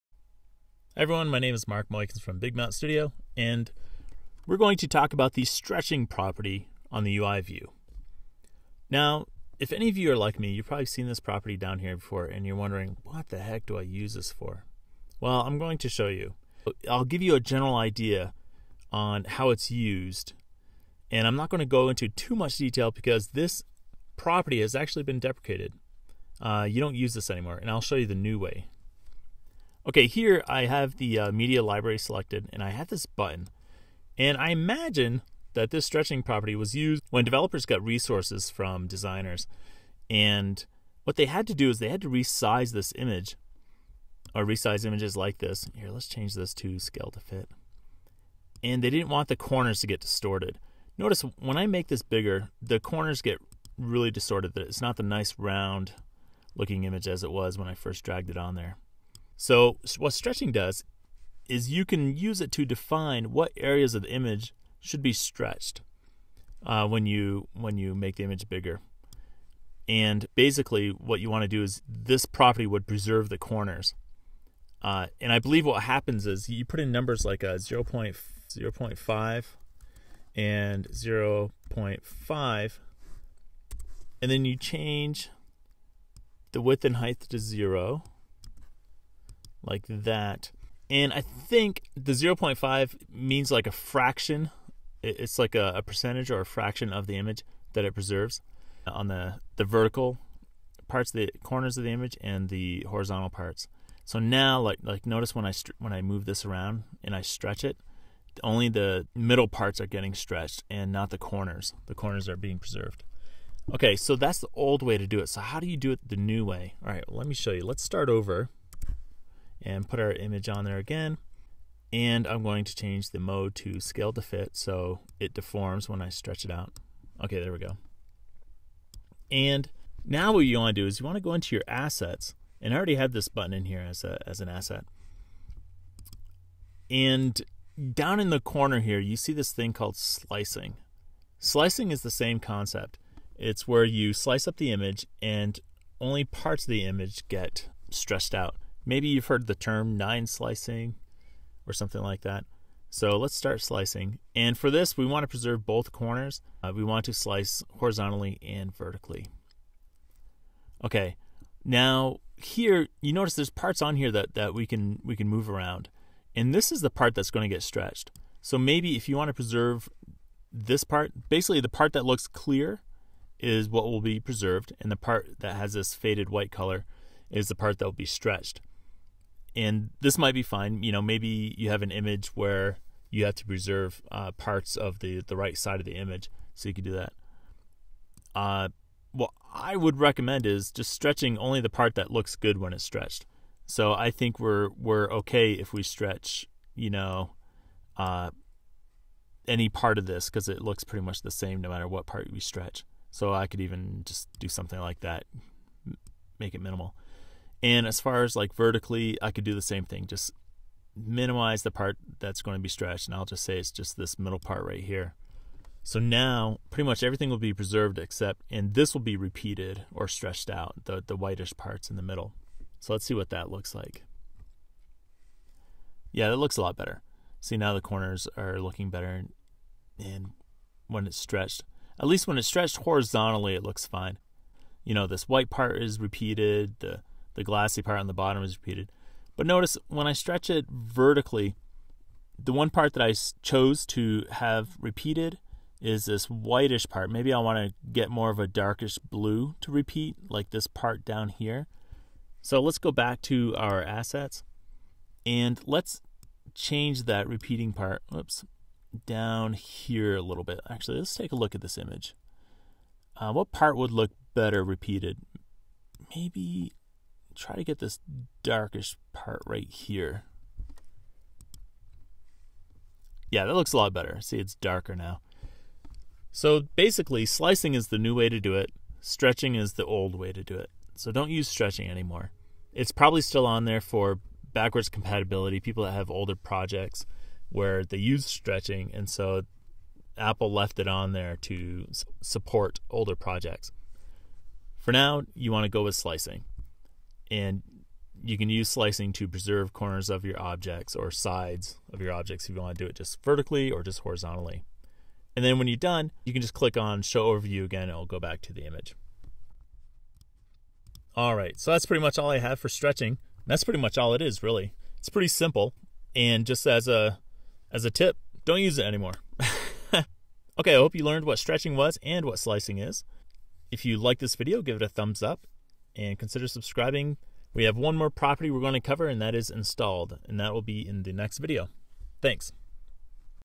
UIView Properties Part 14 - Stretching (iOS, Swift, Xcode 8)
The fourteenth UIView property in the Attributes Inspector: Stretching. Everyone has seen this property but few understand it. Learn what it is and if you should use it or not.